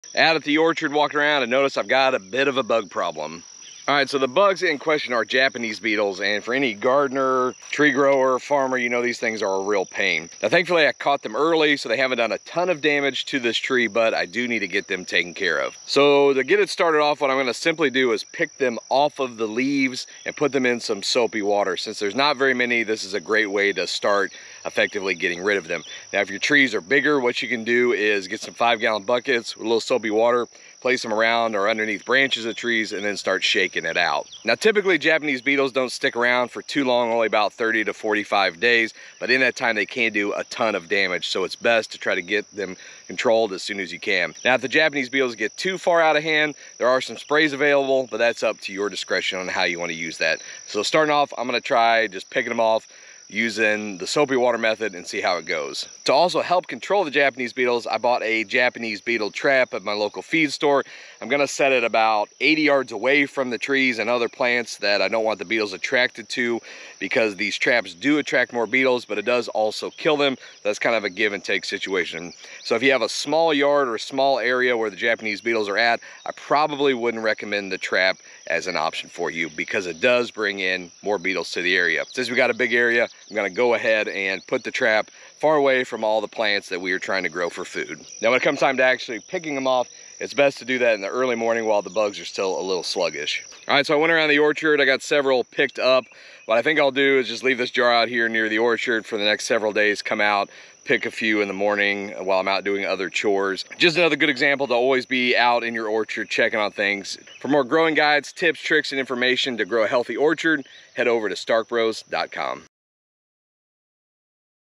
Japanese Beetles and How We're Getting Rid of them in our Orchard
The dreaded Japanese Beetle. They are a serious pest of fruit trees, berry plants, flowers, landscape trees and shrubs, vegetables, field crops, and turf. Adults are said to feed on more than 300 plant species, while the grubs feed mainly on the roots of grasses.

Adult Japanese Beetles leave a lace-like, or "skeletonized" leaf in their wake. Healthy and established plants and trees usually come away with only cosmetic damage, but smaller vegetation may experience stunted growth, injury, or can even be killed due to persistent feeding.

While the lifespan of an adult Japanese Beetle is only 30-45 days, the females have a high reproductive rate, laying eggs every 24 - 48 hours.

So how can you defend your trees and plants when there are lots of beetles feasting on them? There are several ways.

Andrew at the Howard Homestead shows us his simple spray-free method of removing them from his orchard and berry patches.

For more extreme infestations, there are different sprays you can consider to help fend off Japanese Beetles